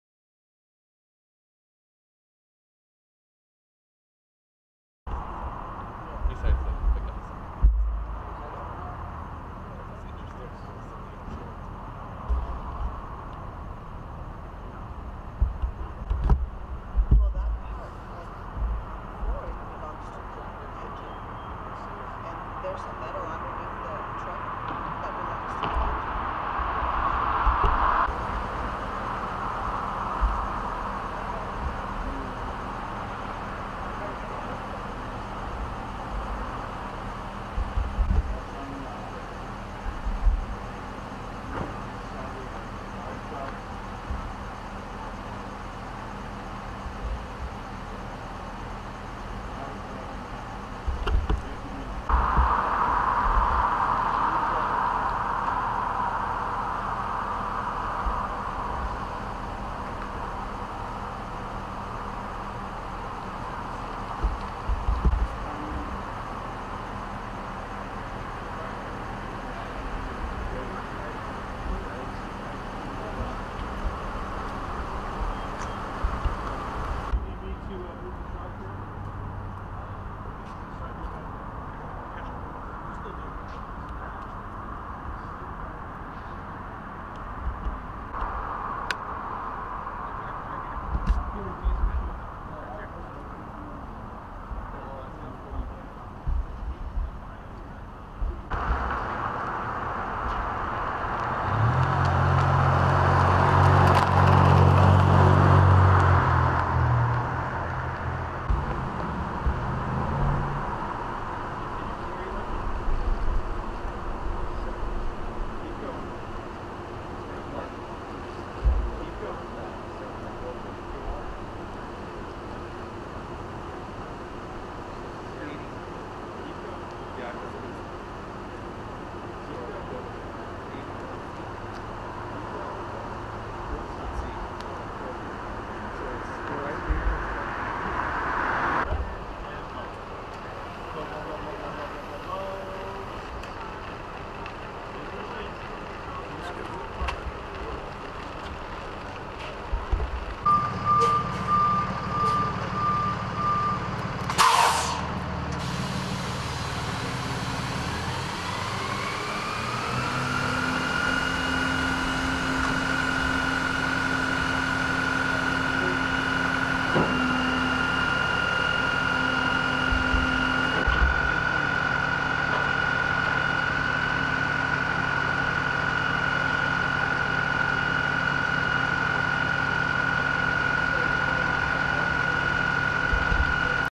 DUI Crash Into Fire Truck Whittier 10 26 19
Shortly before 2:00 am a truck travelling north on Santa Gertrudes Ave. collided with a LACo fire truck in front of its fire station in the city of Whittier.  CHP responded to a report of a pick up truck that crashed into the side of fire station 15's truck while returning the truck into the station.  The man who was driving the pick up truck was seen taking a field sobriety test and CHP officers took the man into custody on suspicion of DUI.  No firefighters were injured in the crash and the CHP is investigating.